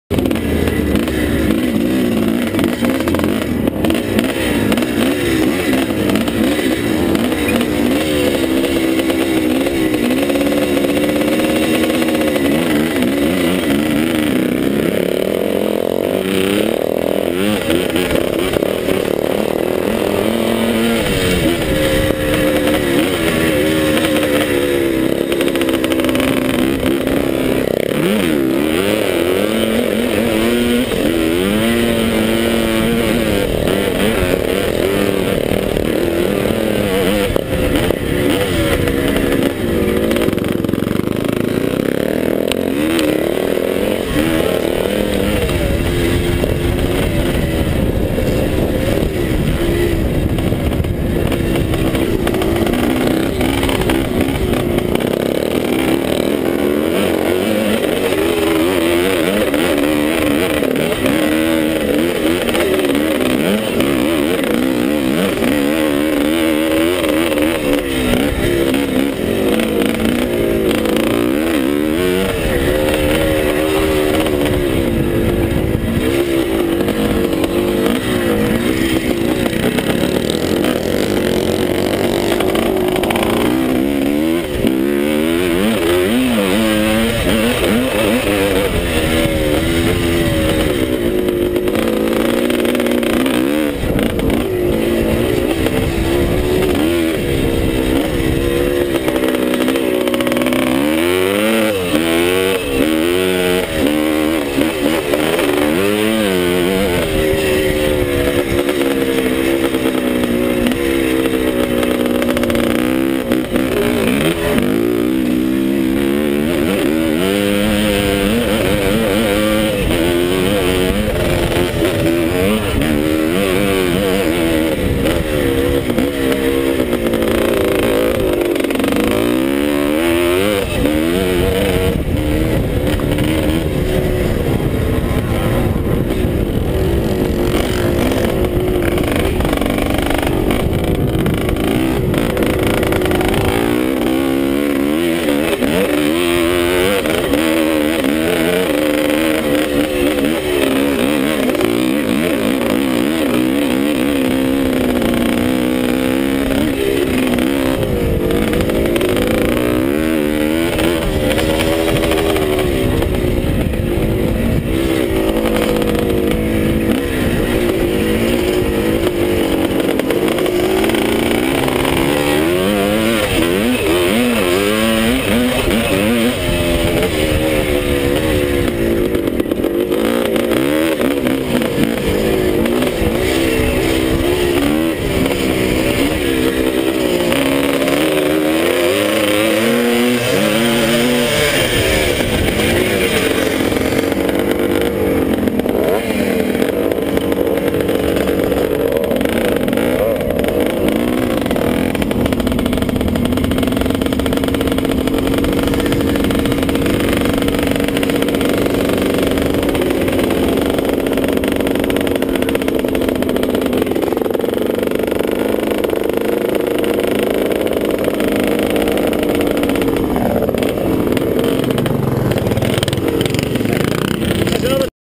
Llanthony Classic Scramble : Twinshock Race3
Mark Evans on his Montesa Cappra for race 3 in the Twinshock class at Llanthony